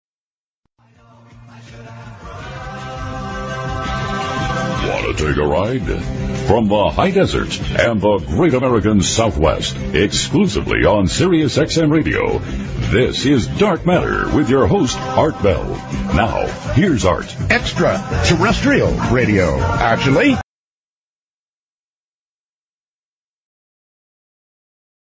Ross Mitchell introducing Art Bell in 2003
Deepest voice on radio